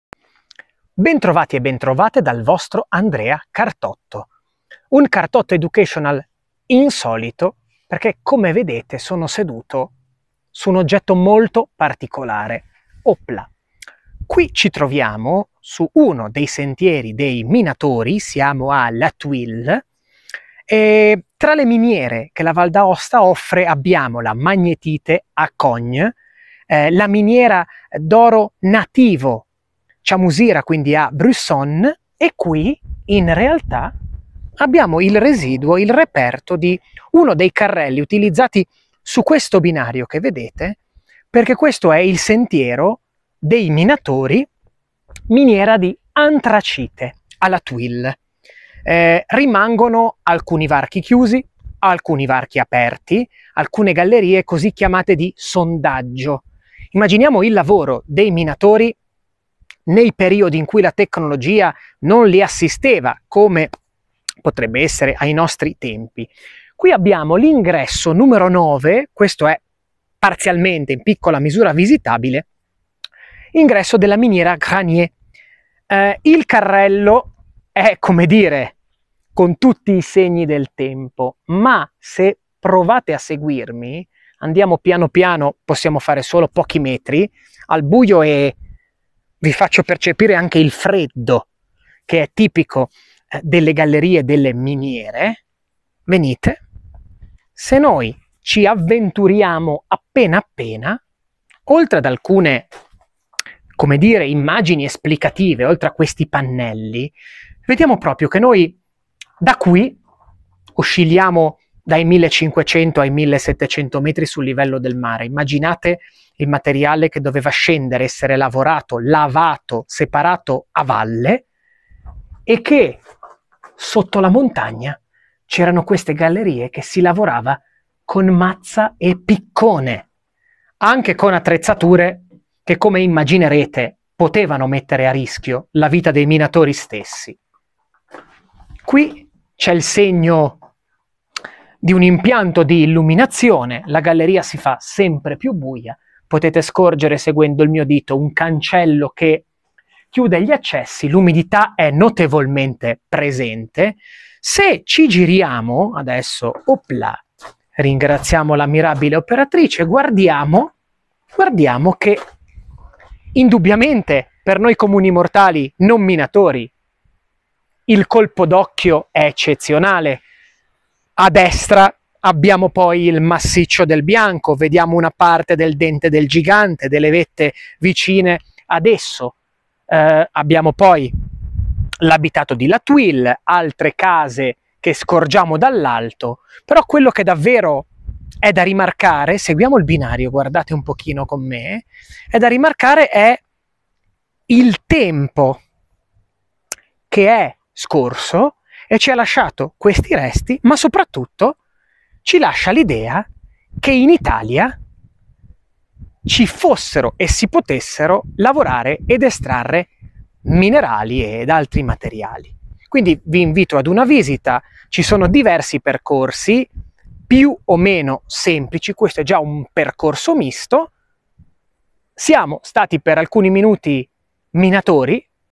Andrea Cartotto - cartottoeducational / Vita da minatori: la Miniera di antracite de La Thuile
Dura, la vita dei minatori italiani degli anni (a volte, secoli) passati: é il caso delle miniere letteralmente "dentro" la montagna, come nel caso della Miniera de La Thuile. Qui si estraevano piombo argentifero ed antracite: tramite i sentieri tuttora esistenti, mi sono recato a cercare e scoprire i resti di una storia aspra e affascinante: ve li mostro e racconto in questo nuovo video.